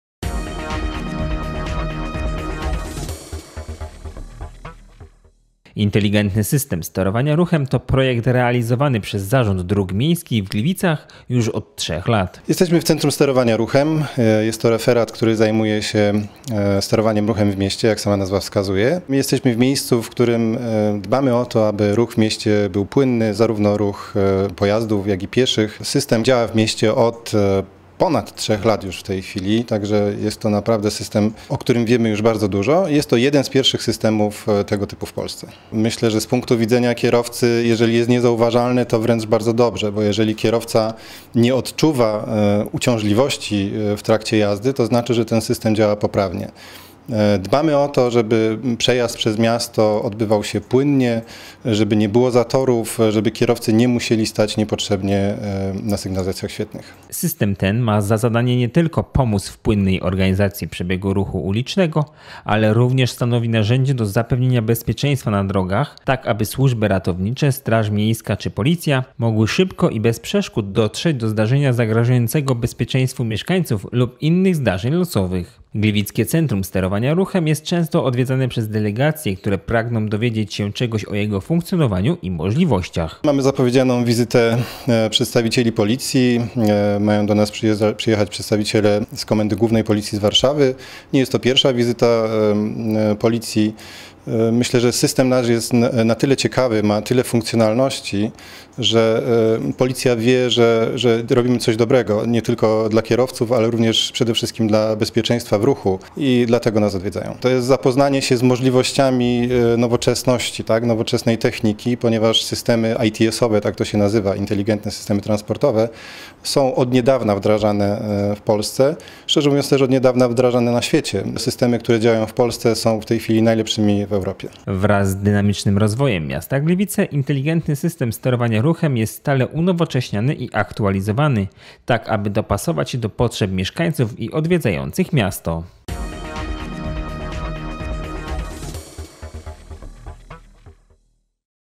W Gliwicach to potrafią
Gliwicki system sterowania ruchem jest jednym z nielicznych w Polsce i jednym z najlepszych w Europie. W środę  działające przy ZDM Centrum Sterowania Ruchem odwiedziła delegacja Komendy Głównej Policji.